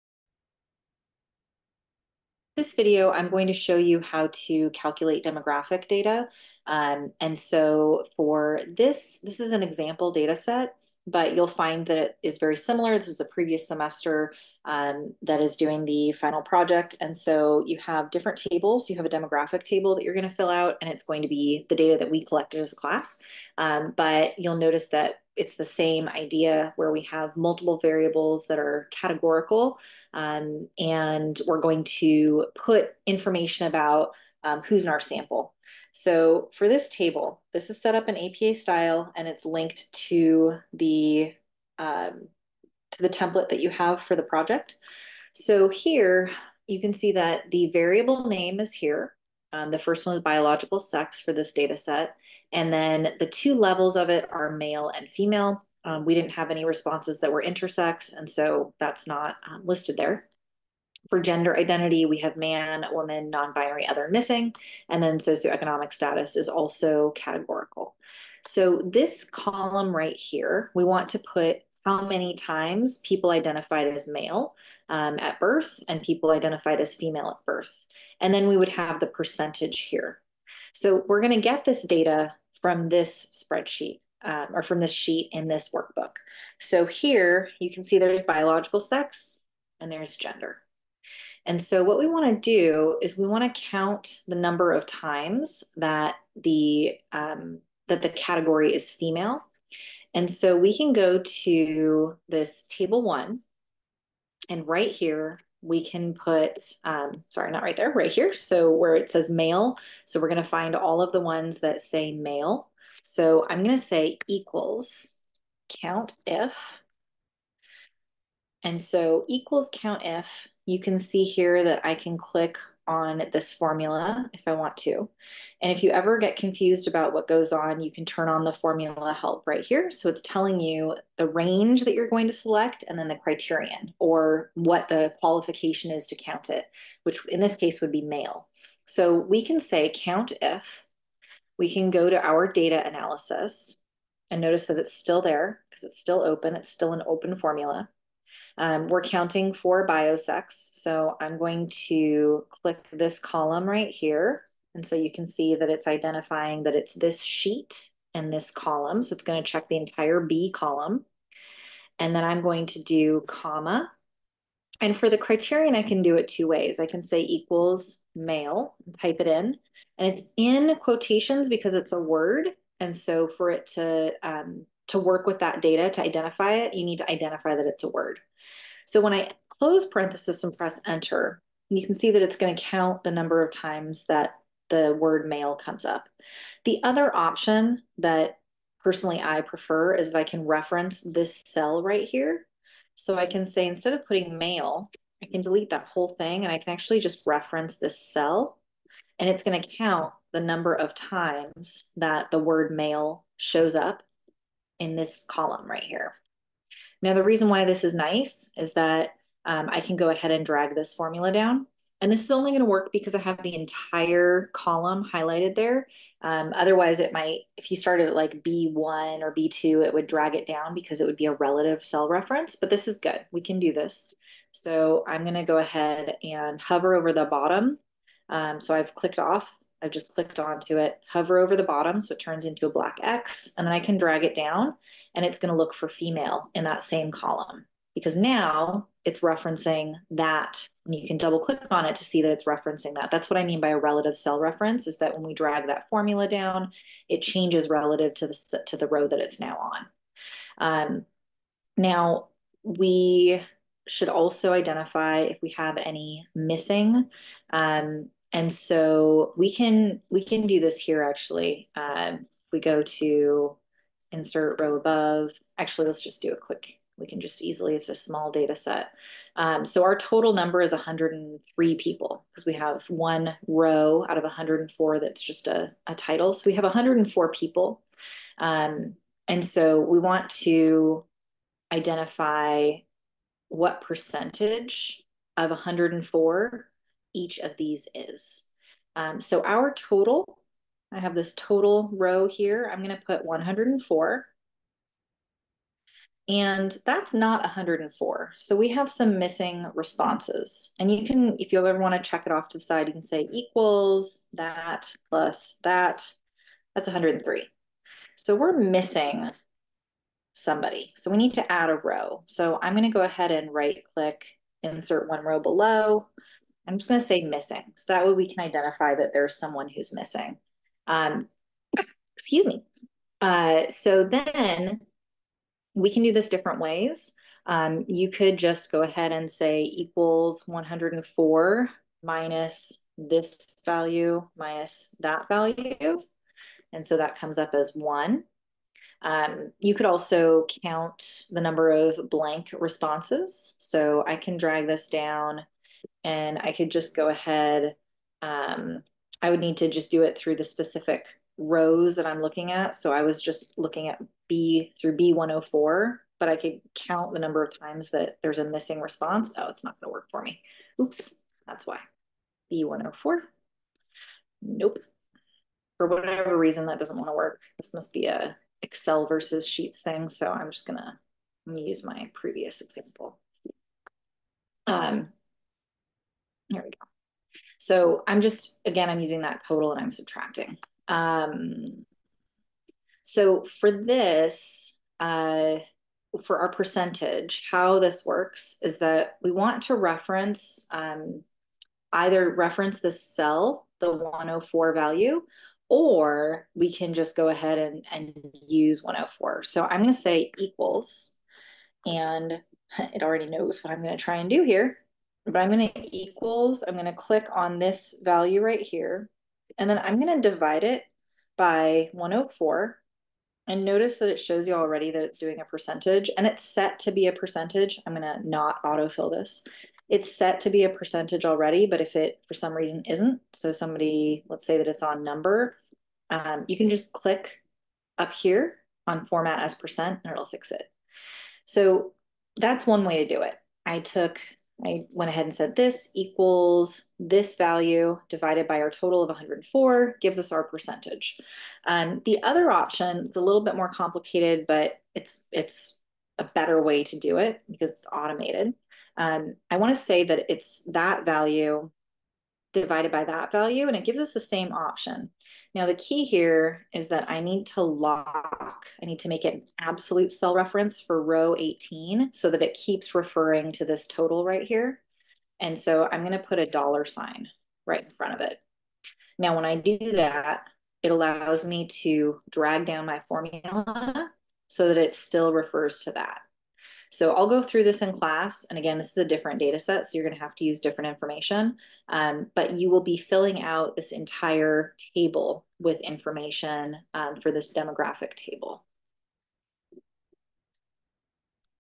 Table 1 - Demographics in 7th Edition APA
This video reviews how to create the first table in your Final Statistical Report for PSYCH 42. In this table, you will include the categorical demographic variables. You will create the table in sheets and then copy it into your report.